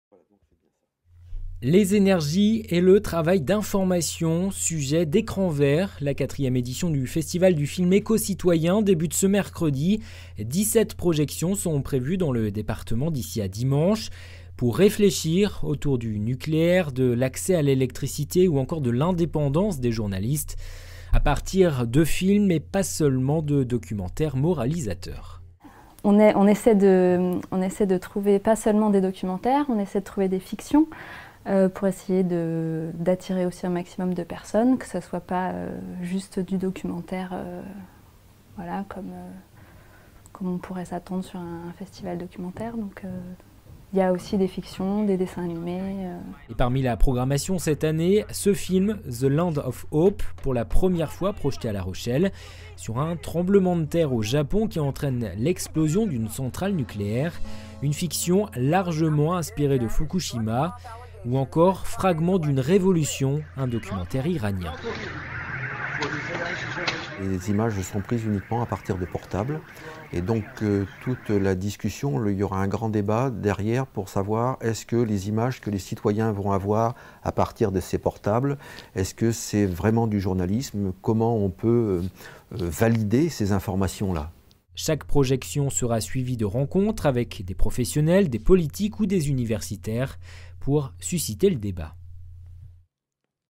Célà tv Tout Images - 4e édition du festival du film éco-citoyen Ecran Vert
Sujet du Tout Images du 01/10/2013
Les énergies et le travail d'information, thèmes d'Ecran Vert. La 4e édition du festival du film éco citoyen débute ce mercredi. 17 projections sont prévues dans le département d'ici à dimanche. Pour réfléchir autour du nucléaire, de l'accès à l'électricité ou de l'indépendance des journalistes.